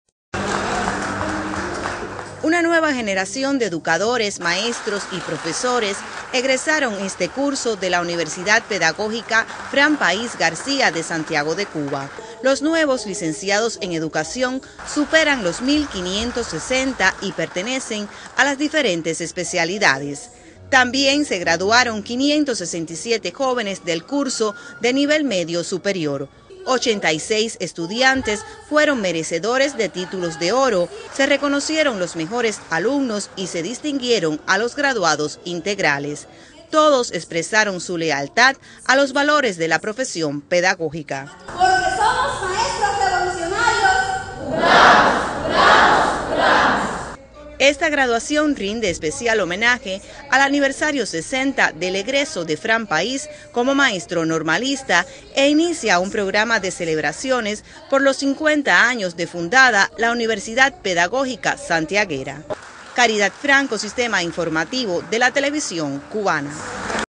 Realiza Universidad Pedagógica graduación 60 aniversario de la gesta del Moncada
La Universidad Pedagógica santiaguera Frank País García realizó la graduación 60 aniversario del Moncada. A la ceremonia asistieron las máximas autoridades del Partido y el Gobierno de la provincia.